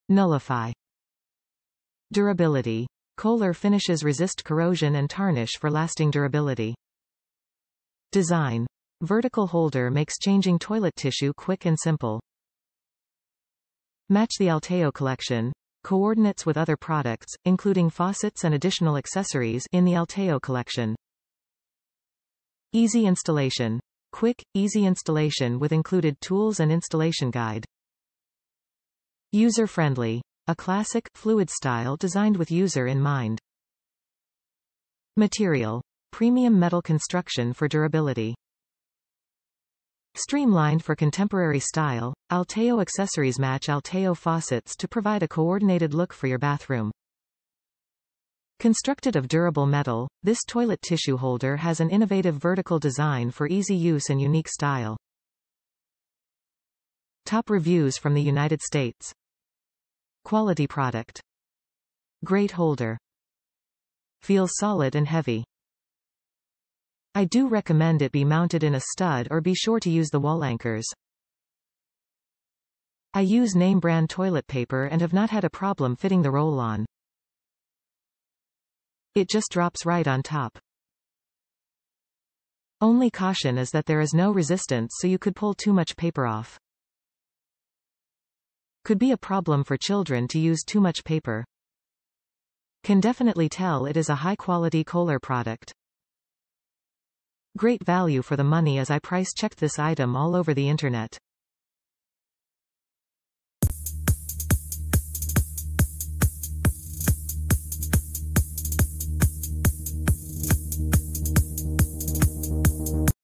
KOHLER K-37056-CP Alteo Vertical Tissue Holder, Polished Chrome
Nullify
DURABILITY: KOHLER finishes resist corrosion and tarnish for lasting durability.
DESIGN: Vertical holder makes changing toilet tissue quick and simple.
MATCH THE ALTEO COLLECTION: Coordinates with other products, including faucets and additional accessories, in the Alteo collection.
EASY INSTALLATION: Quick, easy installation with included tools and installation guide.
USER FRIENDLY: A classic, fluid style designed with user in mind.
MATERIAL: Premium metal construction for durability.

Streamlined for contemporary style, Alteo accessories match Alteo faucets to provide a coordinated look for your bathroom. Constructed of durable metal, this toilet tissue holder has an innovative vertical design for easy use and unique style.

Quality product
Great holder. Feels solid and heavy. I do recommend it be mounted in a stud or be sure to use the wall anchors. I use name brand toilet paper and have not had a problem fitting the roll on. It just drops right on top. Only caution is that there is no resistance so you could pull too much paper off. Could be a problem for children to use too much paper.
Can definitely tell it is a high quality Koehler product. Great value for the money as I price checked this item all over the internet.